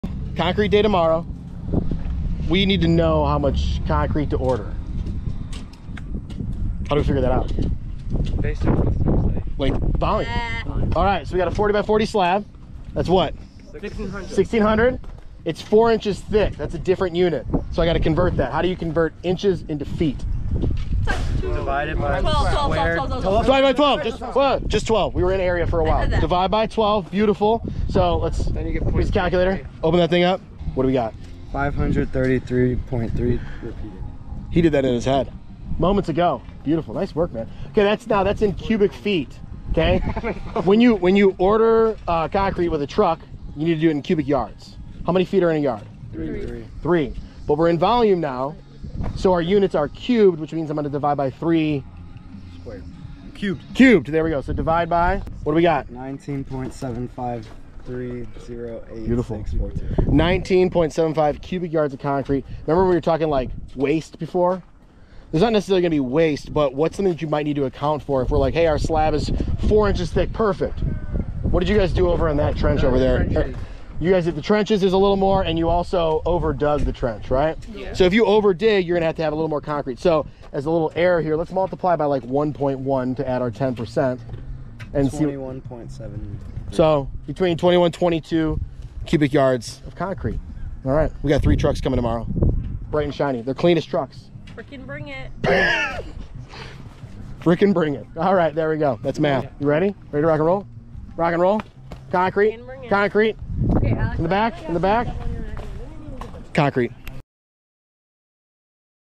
Concrete Day Tomorrow! But How Much Do We Need?! 🏗️🧮
Concrete day is almost here — but first we need to do the math! In this MathInTheWild lesson, we calculate the volume of our slab, convert units, and figure out exactly how many cubic yards of concrete to order from Ozinga.

This is real-world geometry in action: unit conversions, volume formulas, and problem solving with a real construction project.

👷‍♂️ Would your students be able to solve this challenge?